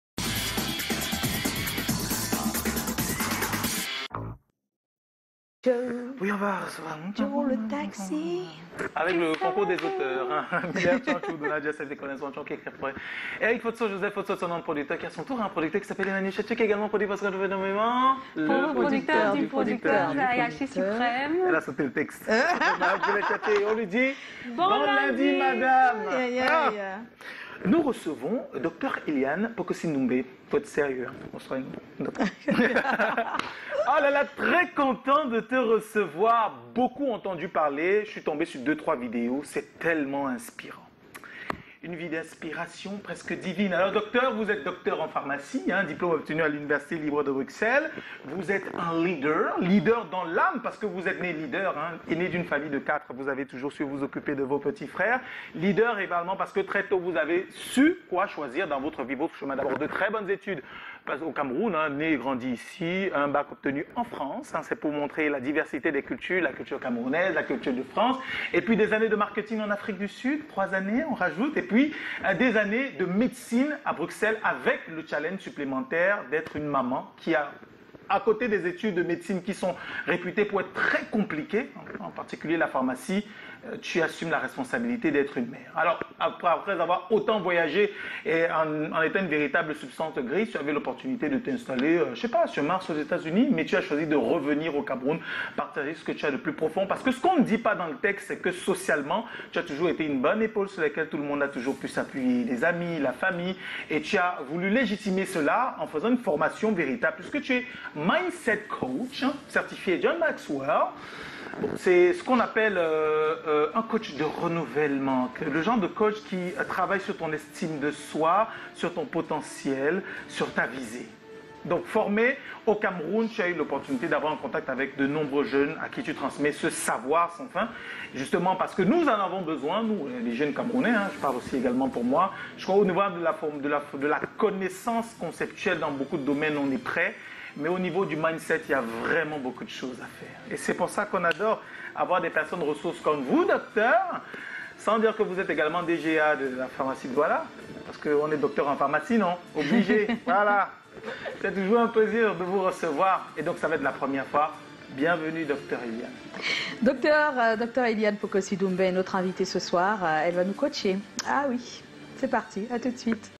Incroyable !!! Un Docteur qui devient coach mindset!
Dr . ELIANE POKOSSY DOUMBE, Coach certifié John Maxwel ( C'comment du Lundi 13/02/2023 )